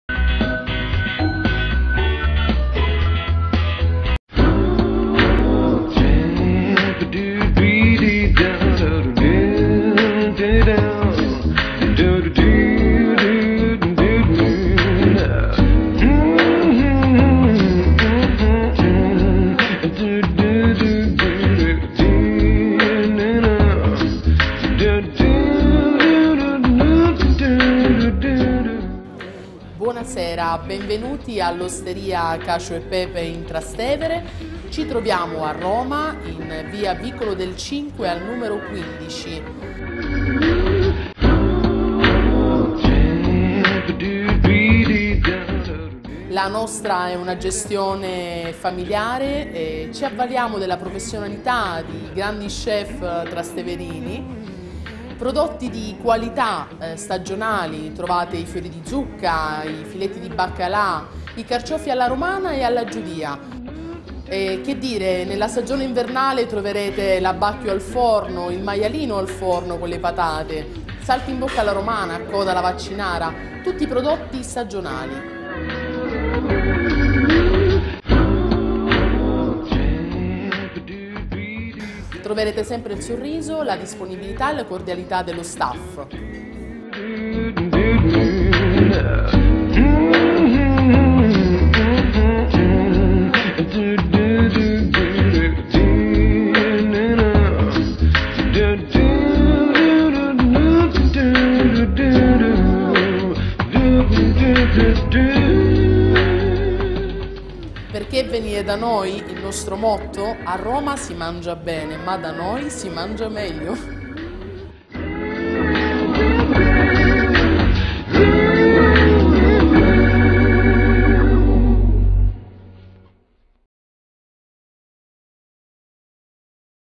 Cacio e Pepe
Quality seasonal ingredients are used in the classic Roman dishes at this charming osteria. The family owners make everyone feel right at home with their generous authentic dishes.